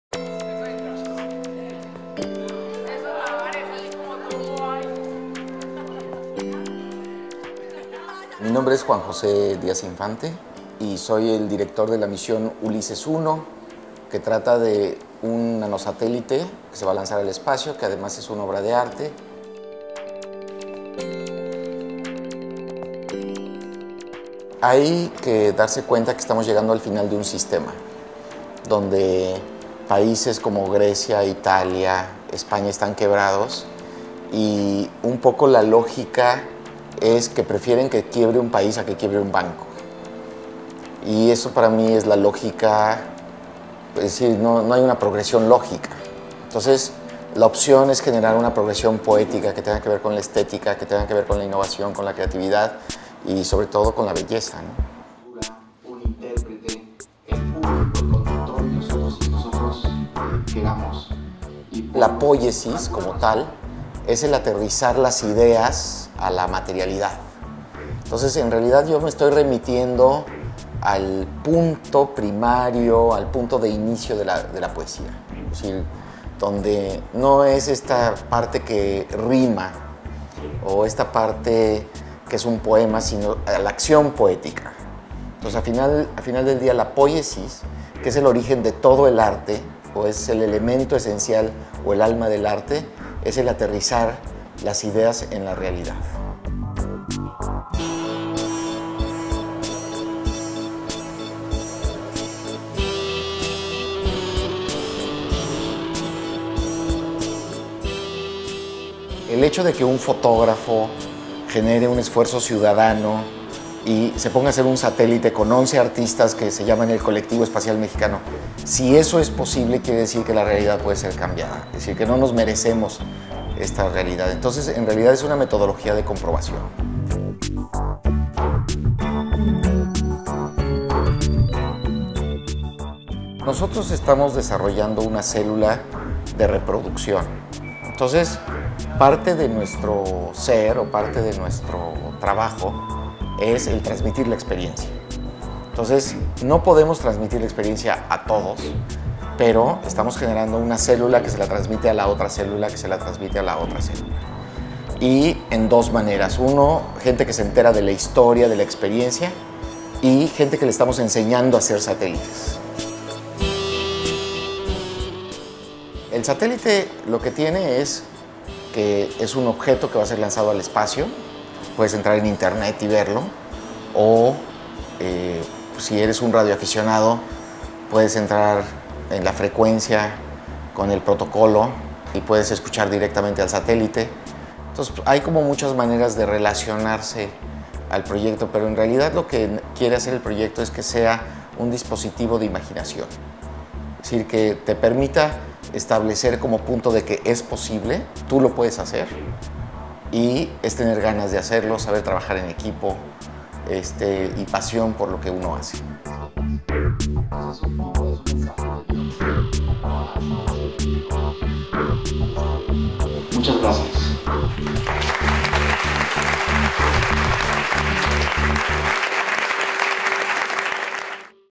Astropoética Nanosatélite Ulises
Crear un satélite y enviarlo al espacio puede ser considerado una obra de arte. Y así es como lo ve un hombre que ha creado uno y lo ha puesto en la órbita de la tierra.

Él, ese creador que ha construido en compañía de otros artistas un satélite y sobre todo una filosofía, estuvo en Medellín y nosotros fuimos a hablar con él sobre eso que realiza y todo lo que puede rodear la construcción y puesta en órbita de éste objeto.